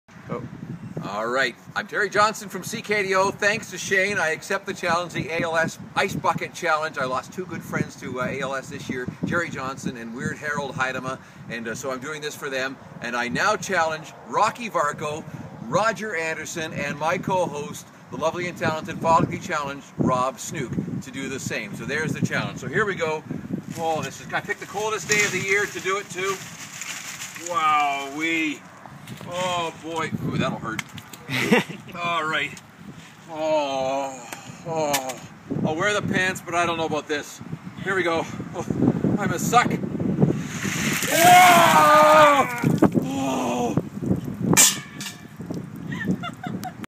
IceBucketChallenge: CKDO's Terry Johnston
He is now challenging Rocky Varcoe, Roger Anderson and Rob Snoek!